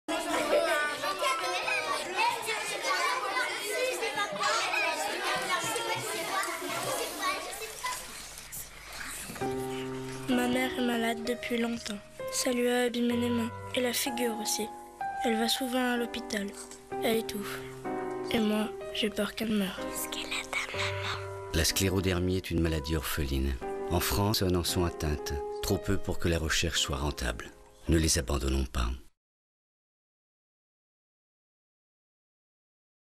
Sensibilisation à la sclérodermie
Clip réalisé par Patrice Leconte pour faire connaître la sclérodermie.
Pour l'Association des Sclérodermiques de France